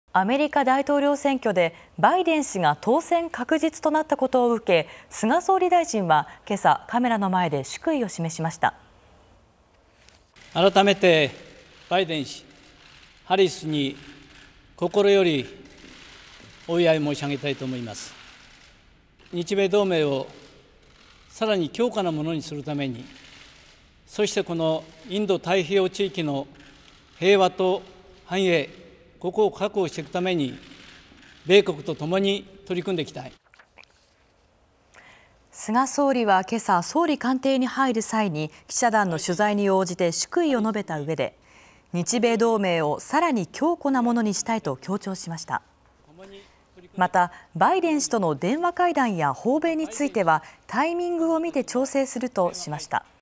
菅総理「日米同盟さらに強固なものに」（2020年11月9日）
アメリカ大統領選挙でバイデン氏が当選確実となったことを受け菅総理大臣はけさ、カメラの前で祝意を示しました。
菅総理「改めてバイデン氏、ハリス氏に心よりお祝い申し上げたいと思います。日米同盟をさらに強固なものにするためにインド太平洋地域の平和と繁栄、ここを確保していくために米国と共に取り組んでいきたい」
菅総理は総理官邸に入る際に記者団の取材に応じて祝意を述べた上で、日米同盟をさらに強固なものにしたいと強調しました。
またバイデン氏との電話会談や訪米については、タイミングを見て調整するとしました。